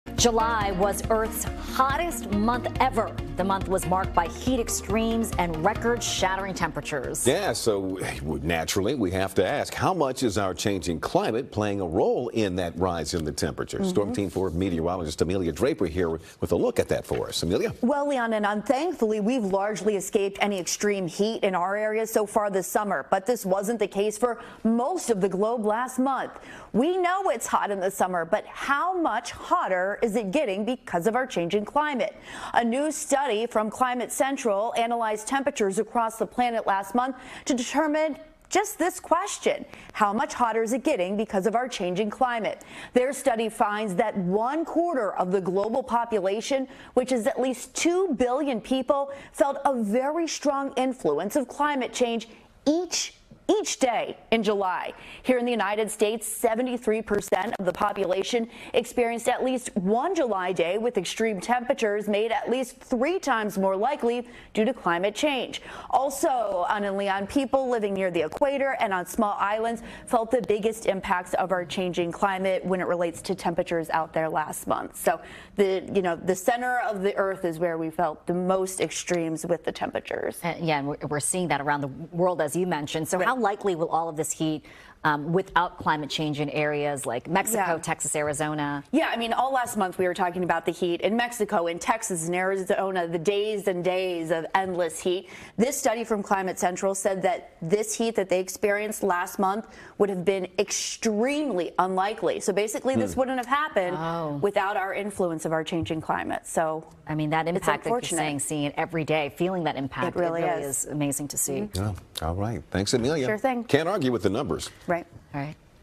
July was Earth's hottest month ever | NBC4 Washington
July was marked by heat extremes and record-shattering temperatures. Storm Team4’s Amelia Draper explains the data.